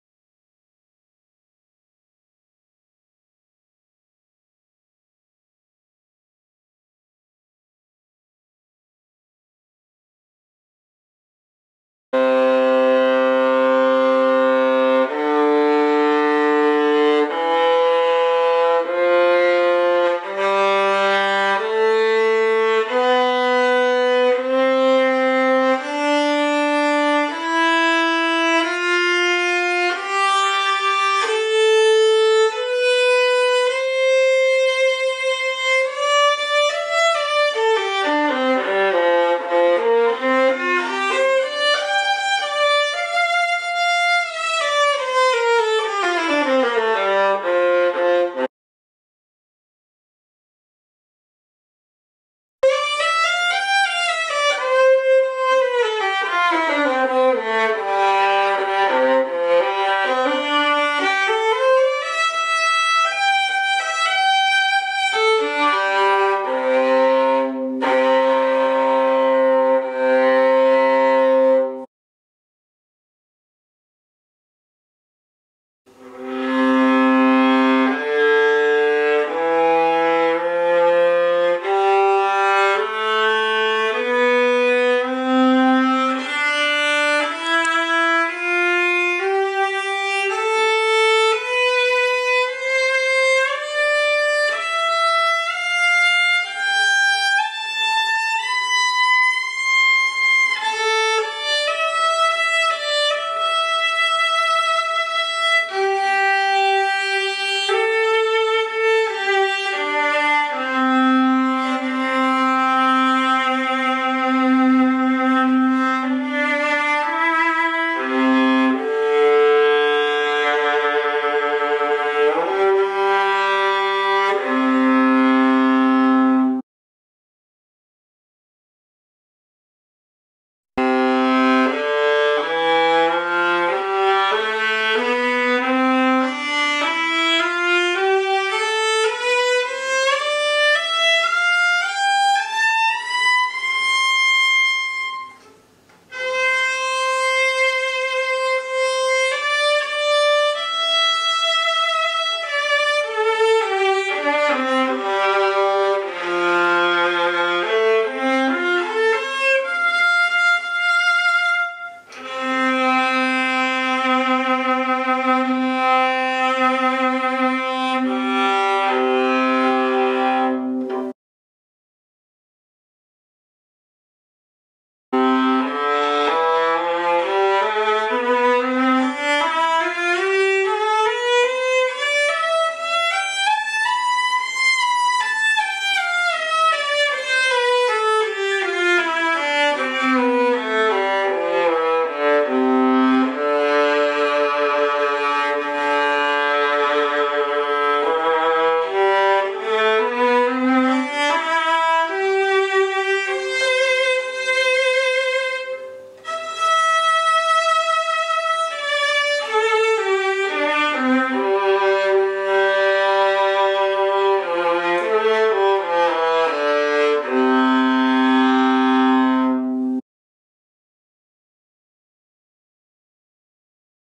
46675ABF 092F 4F89 8FF9 4170F5ECE25A HQ
Strings by Pirastro and D'Addario tested on small size violas (13 1/4 inch and 14 1/4 inch) made by Helen Michetschlager. The player is Lucy Nolan.
The strings are, in order:
Pirastro Tonica 1/2 - 3/4 size on 13 1/4 inch viola
D'Addario Prelude extra short on 13 1/4 inch viola
D'Addario Zyex short on 14 1/4 inch viola
D'Addario Forza on 14 1/4 inch viola
Pirastro Tonica 1/2 - 3/4 size on 14 1/4 inch viola
(13 1/4 inch viola is equivalent to a three-quarter size violin, 14 1/4 inch viola equivalent to a full-size violin.)
For more information and a blog post about these string trials,